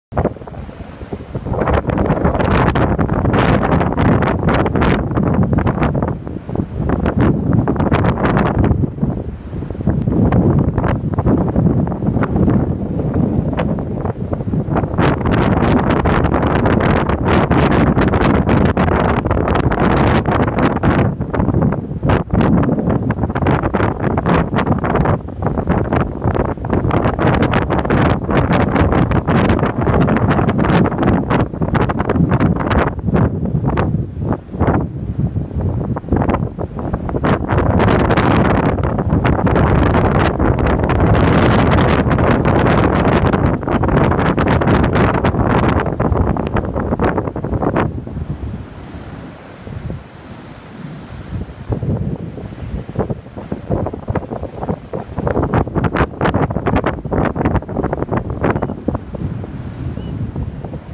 Old Orchard Beach, Maine Nor' Easter April, 2021 near The Pier
Small storm by our famous beach in April of 2021.  Video is shot from the North side of the Pier.